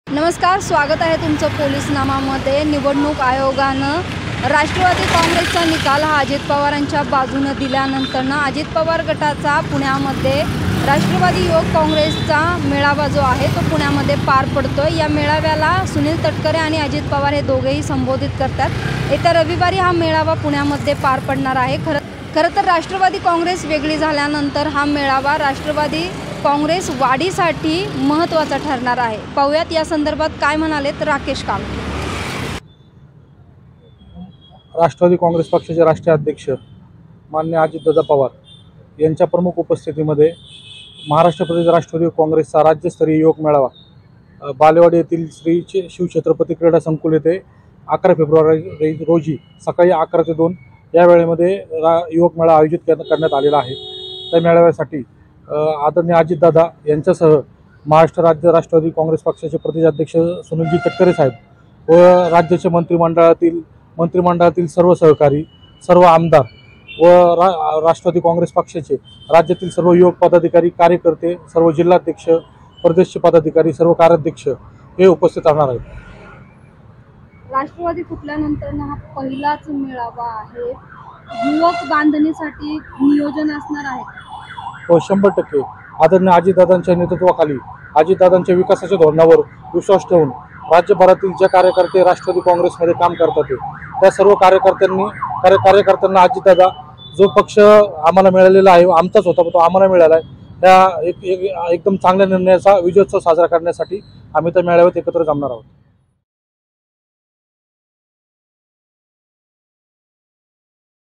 Pune News | निवडणूक आयोगाचा निकाल अजित पवार यांच्या बाजूने लागल्यानंतर पुण्यात राष्ट्रवादी
Pune News | निवडणूक आयोगाचा निकाल अजित पवार यांच्या बाजूने लागल्यानंतर पुण्यात राष्ट्रवादी युवक काँग्रेसचा पहिला मेळावा होणार...